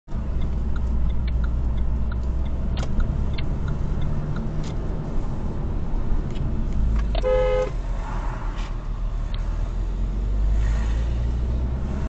Car In Front Slams On Due To Impatient Audi
Car in front of me has to brake due to an impatient Audi driver trying to jump the queue for the roundabout.